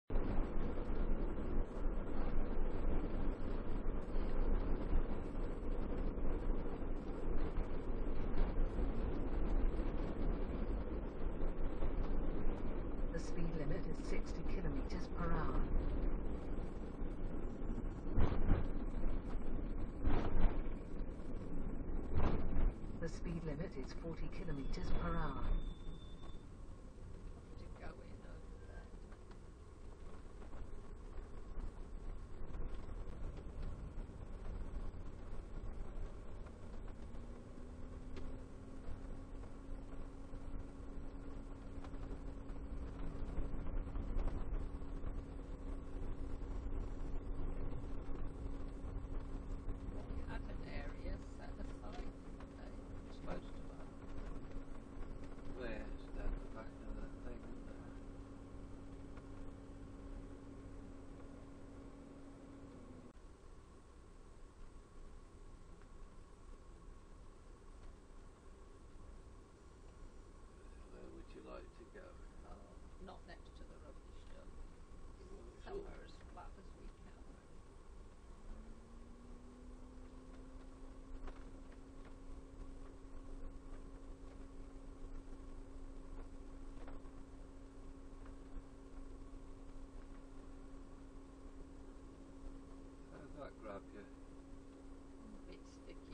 AIRE VALVERDE Spain (on the road between Valencia and Zarragossa
More a lorry park than a aire but safe. With motorhome service point.